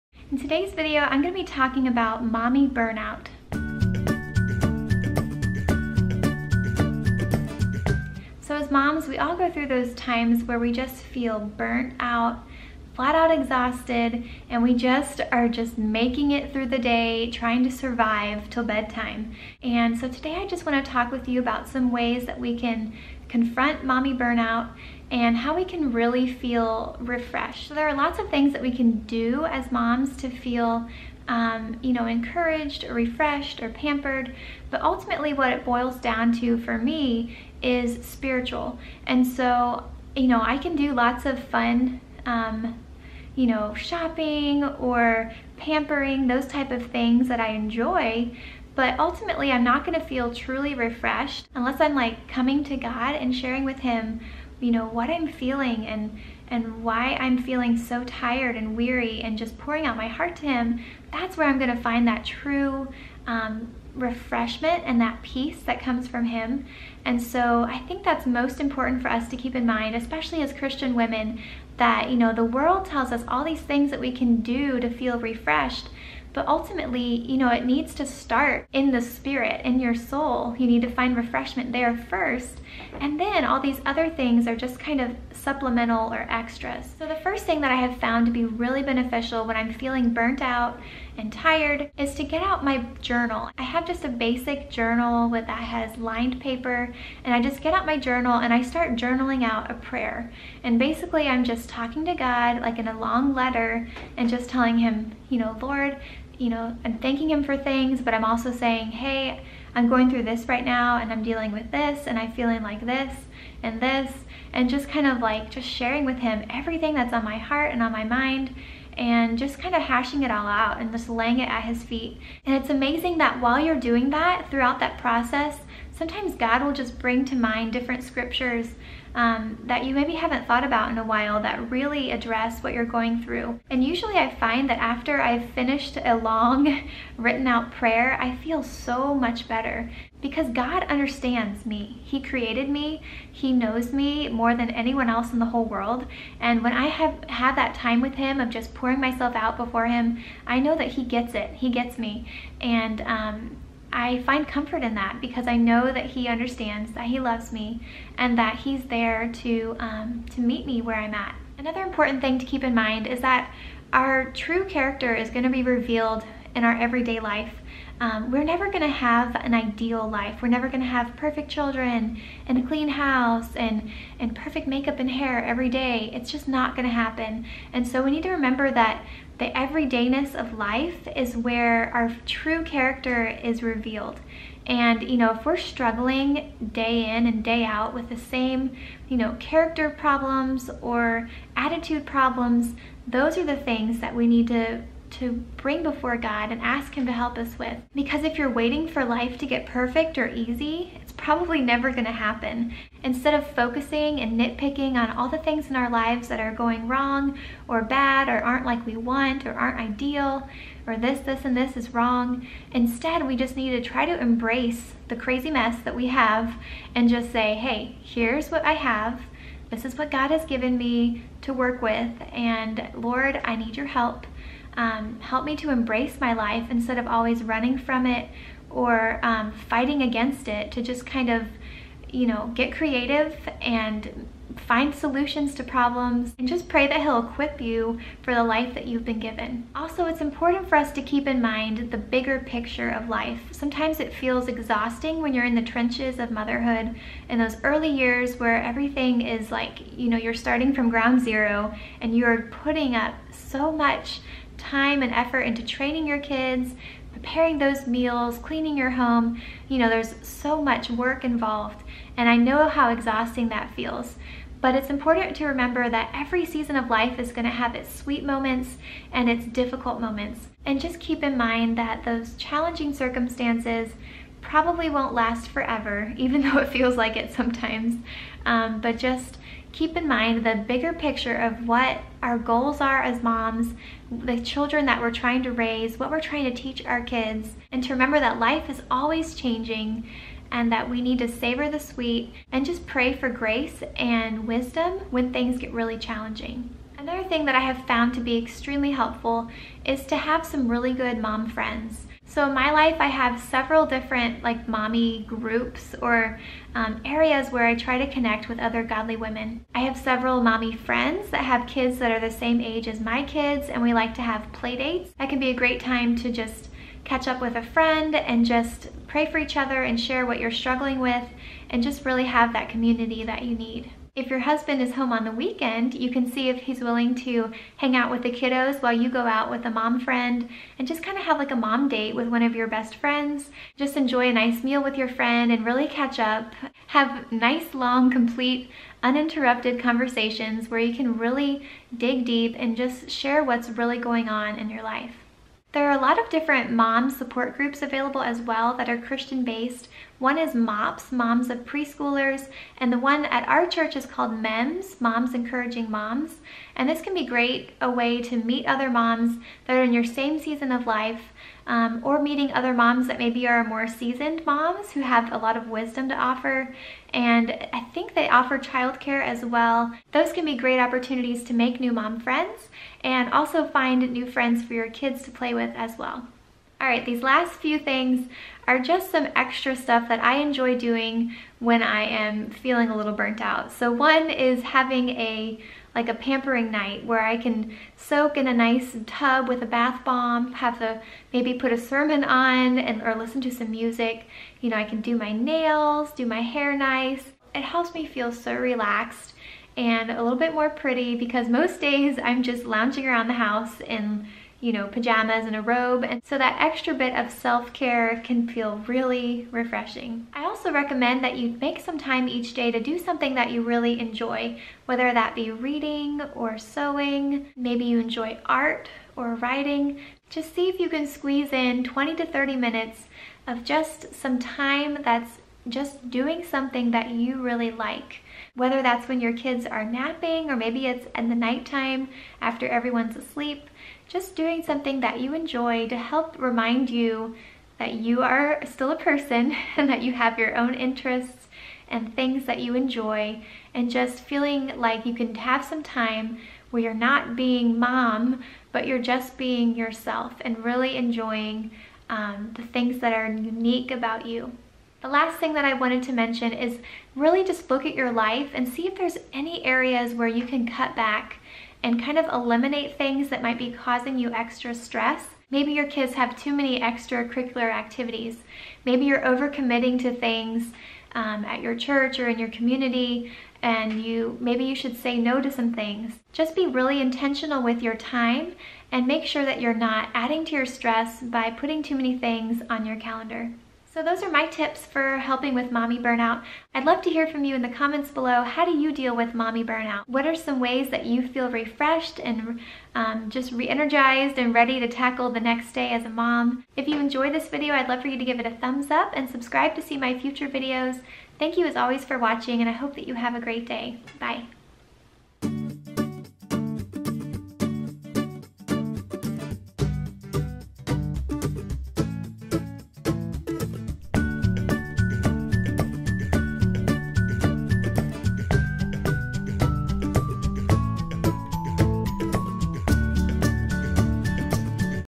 HELP WITH MOMMY BURNOUT
What do you do when you don't feel like you have anything else to give? Where can we as Christian women go to find the refreshment and solitude that we crave?

“And let us not grow weary while doing good, for in due season we shall reap if we do not lose heart.” Galatians 6:9

“Cast your cares on the Lord and he will sustain you; he will never let the righteous be shaken.” Psalm 55:22

“I will sing of the mercies of the Lord forever; With my mouth will I make known Your faithfulness to all generations.” Psalm 89:1

“And He said to me, “My grace is sufficient for you, for My strength is made perfect in weakness.” Therefore most gladly I will rather boast in my infirmities, that the power of Christ may rest upon me. Therefore I take pleasure in infirmities, in reproaches, in needs, in persecutions, in distresses, for Christ’s sake. For when I am weak, then I am strong.” 2 Corinthians 12:9-10